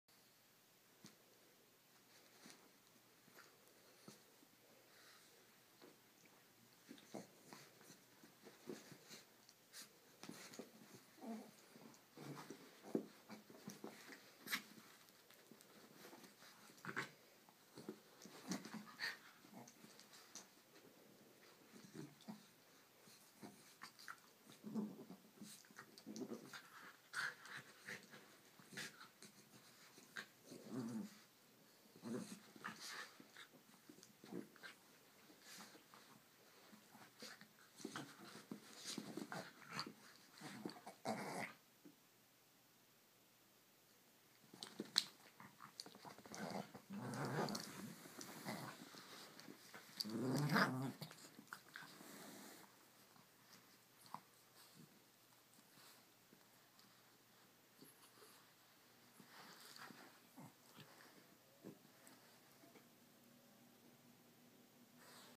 Annie the Shih Tzu and Waldo the mini Schnauzer
Annie and Waldo playing